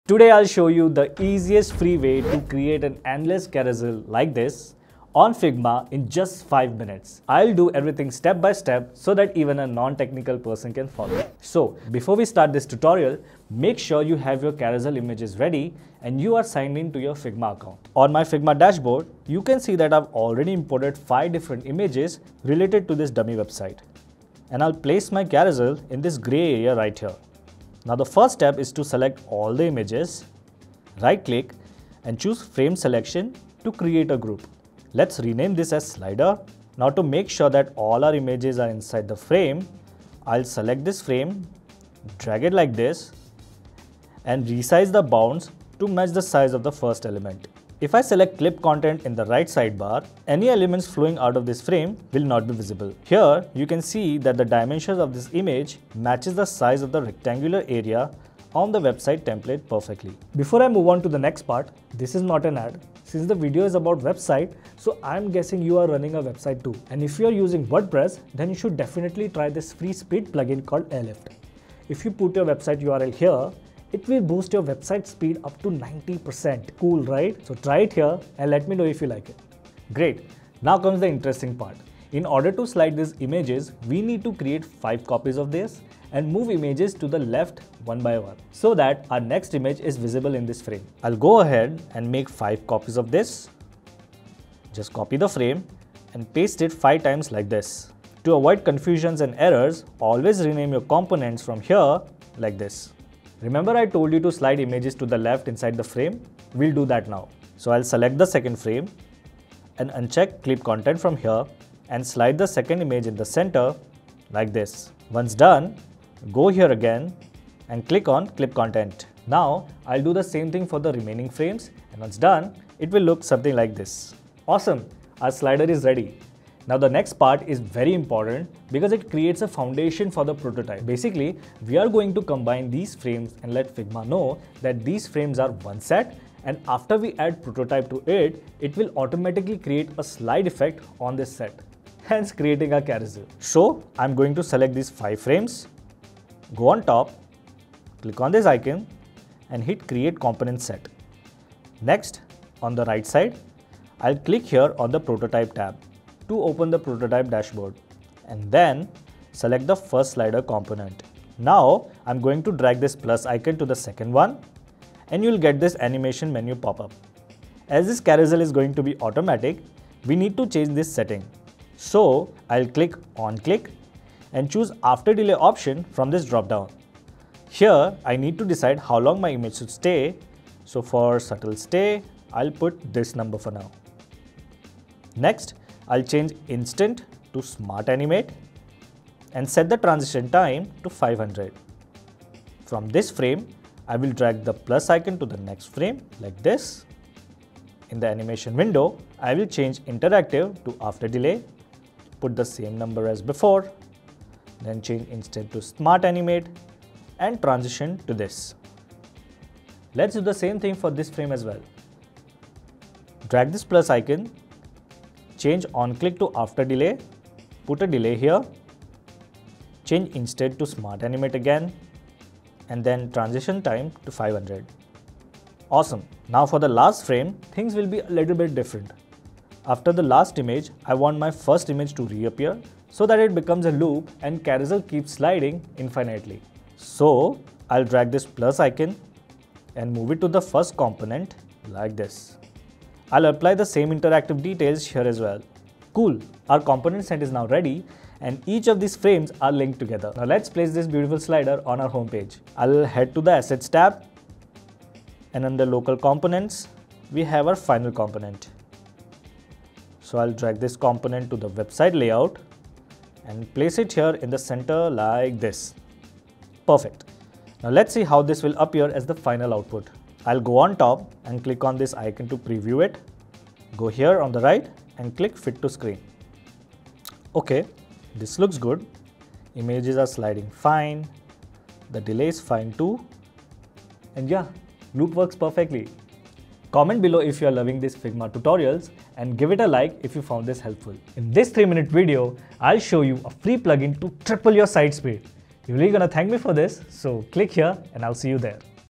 Easiest Automatic Carousel - Figma Tutorial
⭐ Giveaway: Our 100% FREE plugin makes any site 3X faster In this video, I will show you how to create an automatic carousel in Figma.

00:00 How to create automatic carousel?
00:28 Create a group for slider
01:00 DID YOU KNOW ABOUT THIS?
01:31 Create component set
02:34 Prototype
04:06 Create Slider
04:51 BONUS